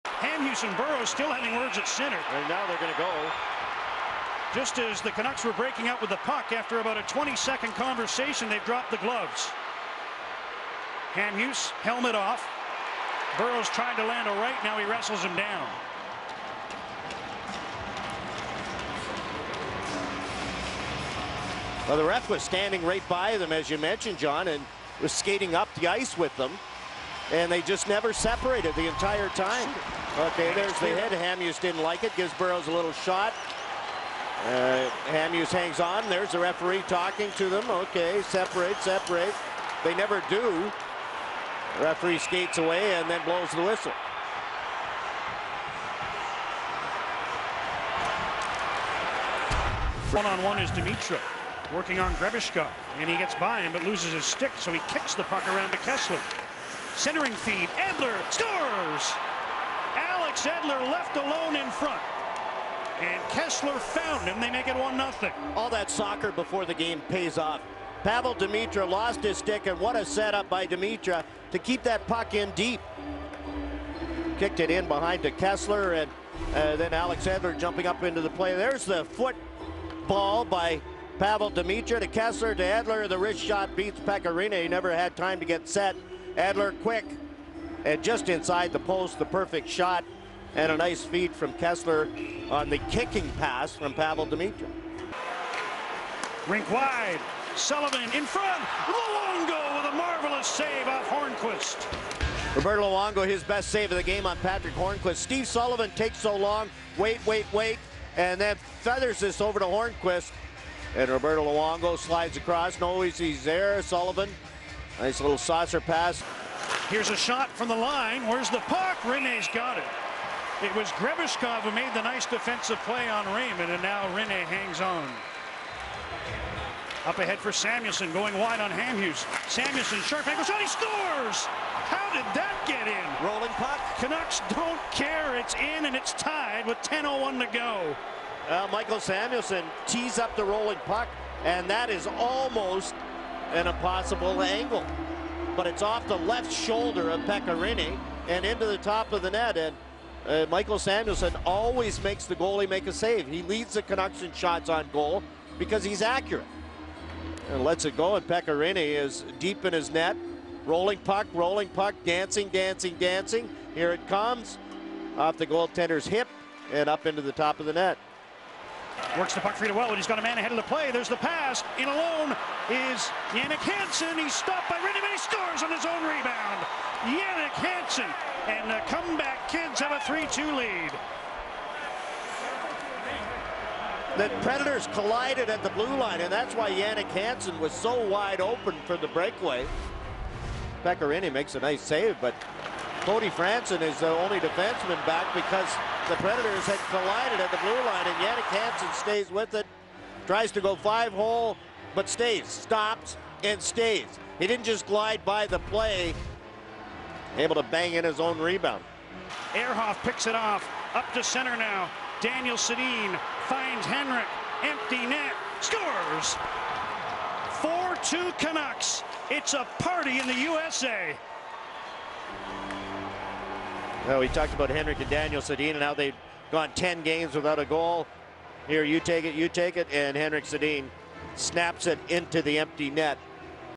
Canucks At Predators - Game Highlights - 03.07.10 - HD
The Vancouver Canucks take on the Nashville Predators. This highlight package includes: Four Goals, One Fight and One Save.